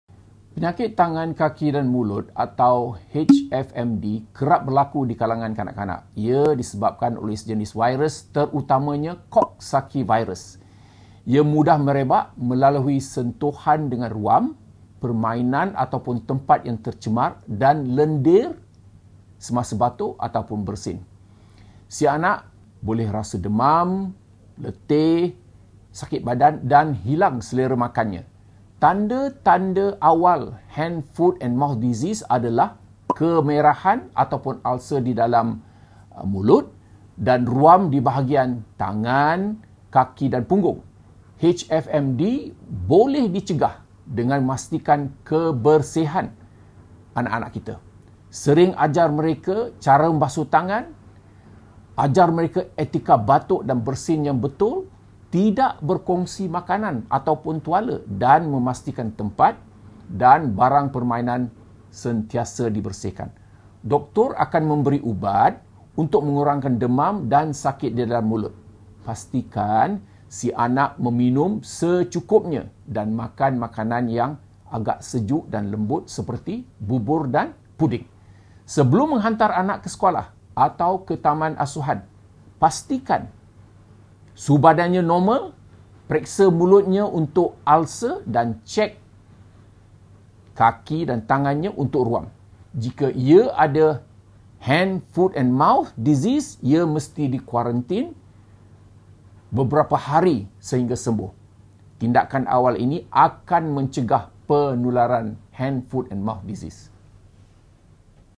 Cara mencegah Penyakit Tangan, Kaki & Mulut - Hand Foot & Mouth Disease (HFMD)
Sebelum anak ke-sekolah/pusat asuhan check:
✅Suhu badan
✅Mulut tiada ulser
✅Tangan/Kaki tiada ruamIsolasi jika ada HFMD
✅Cegah penularan HFMD@MPAEDS @imaret_my @FIMAweb